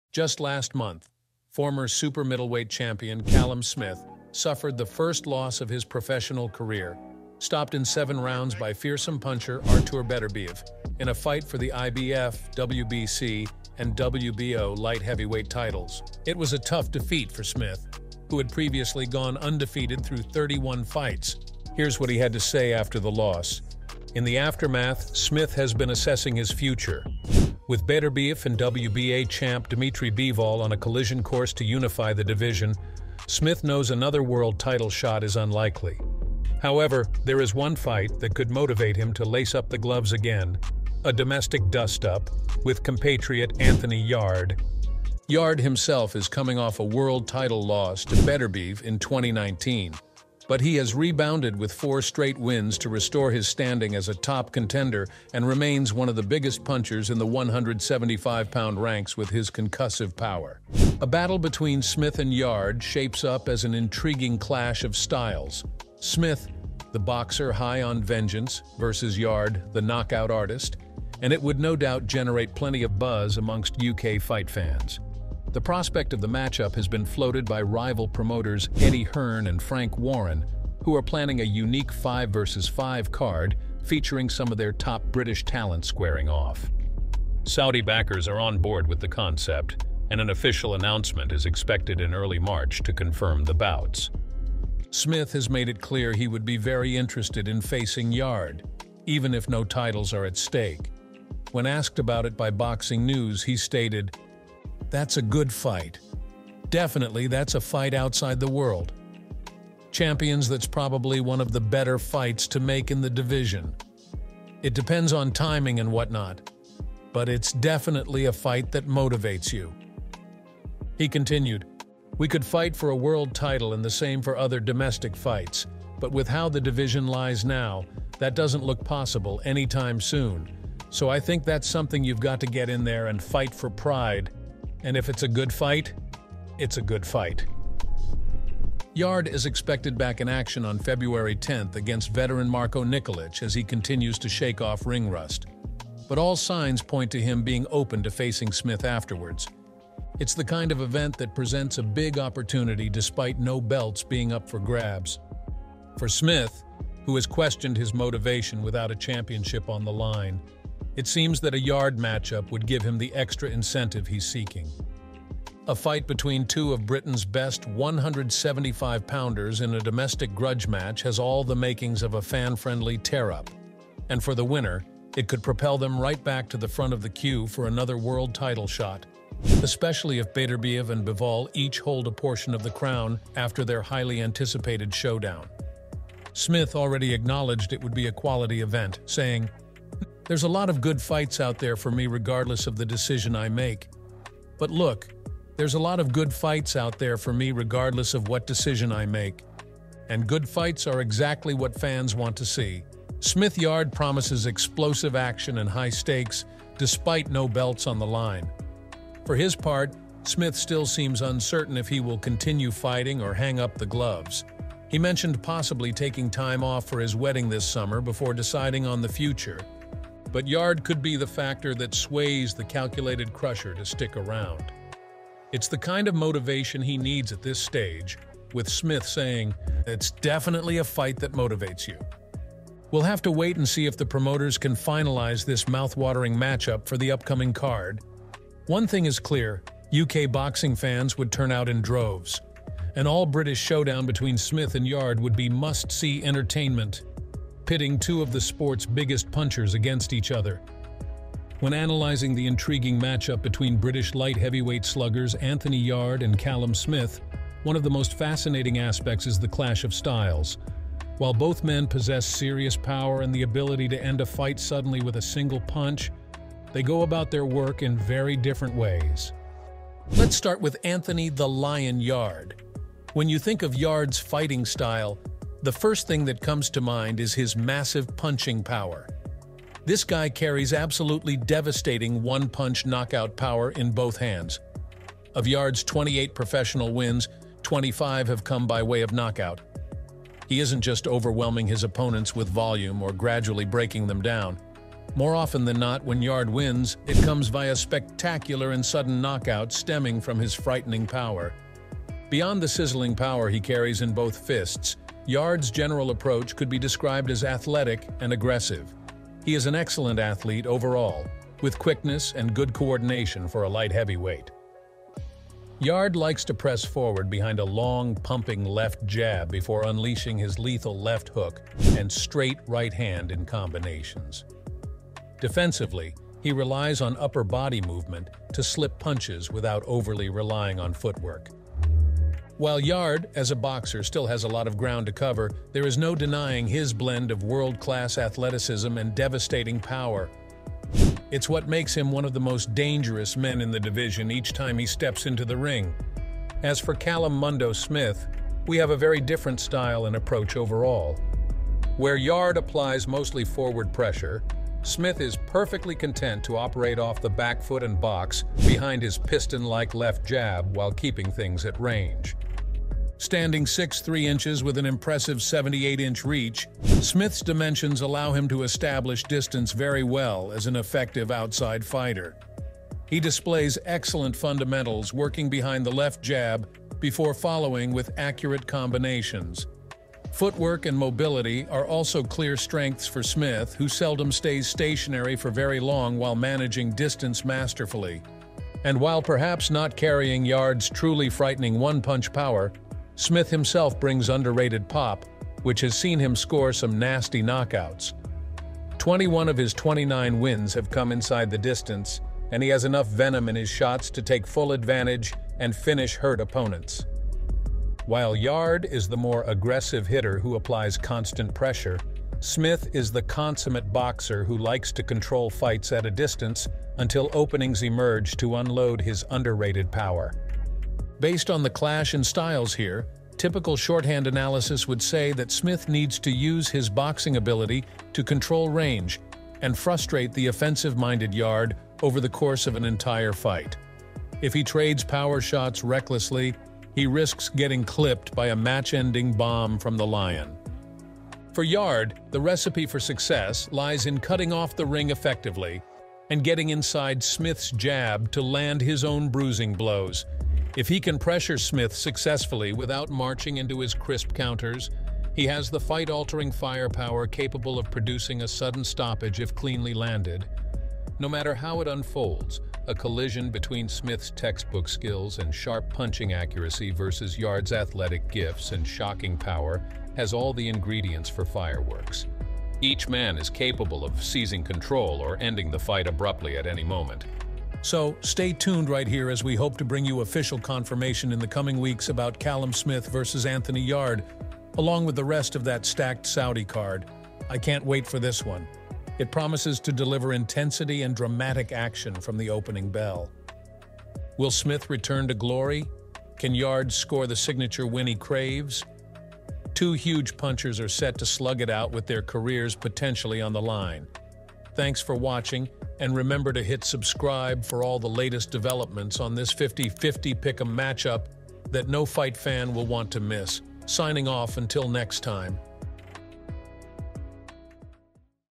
1 MINUTE AGO: Callum Smith Sends FIGHT CONTRACT to Anthony Yarde
1 MINUTE AGO: Callum Smith Sends FIGHT CONTRACT to Anthony Yarde

Just last month, former super middleweight champion Callum Smith suffered the first loss of his professional career, stopped in 7 rounds by fearsome puncher Artur Beterbiev in a fight for the IBF, WBC and WBO light heavyweight titles. It was a tough defeat for Smith, who had previously gone undefeated through 31 fights.
In the aftermath, Smith has been assessing his future. With Beterbiev and WBA champ Dmitry Bivol on a collision course to unify the division, Smith knows another world title shot is unlikely. However, there is one fight that could motivate him to lace up the gloves again – a domestic dust-up with compatriot Anthony Yarde.